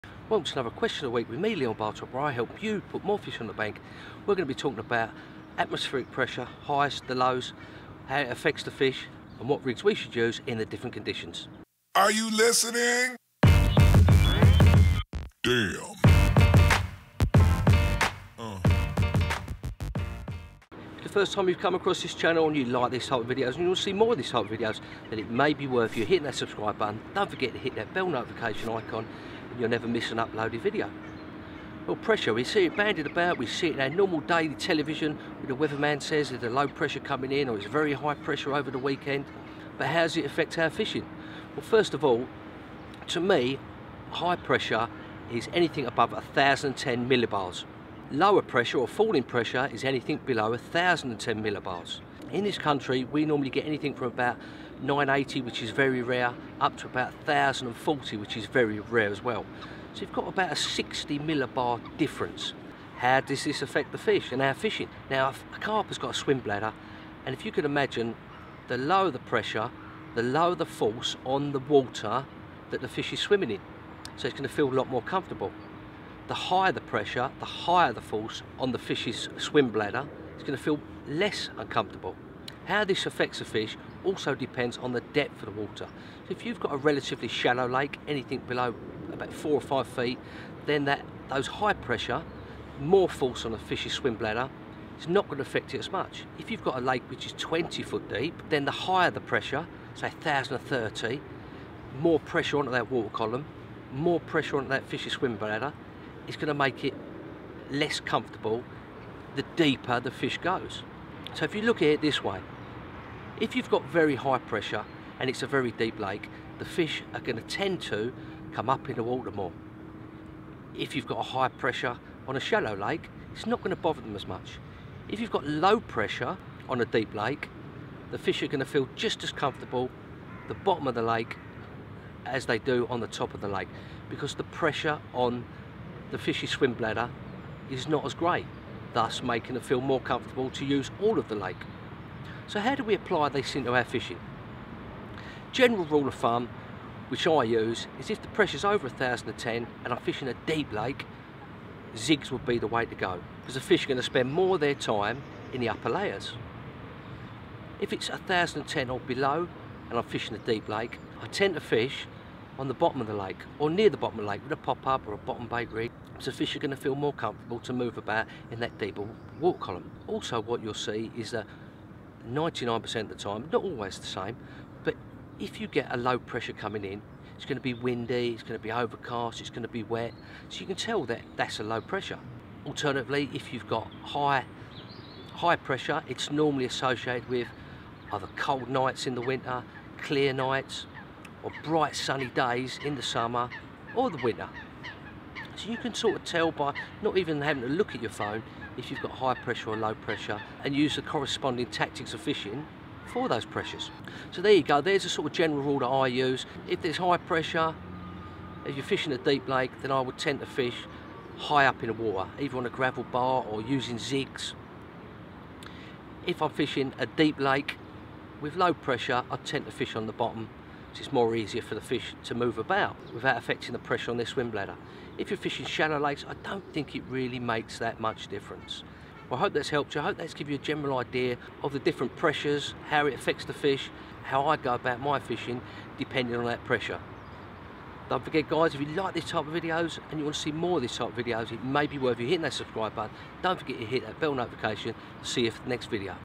HOW DOES WEATHER AFFECT CARP FISHING ? 😀
HOW DOES WEATHER AFFECT CARP FISHING ? 😀 - Ever wanted to know how carp react to different weather pressures and what rigs and tactics to use then join me as I run through both High Pressure and Low Pressure.

🔴 LIVE STREAM Q&A EVERY TUESDAY & SATURDAY!!!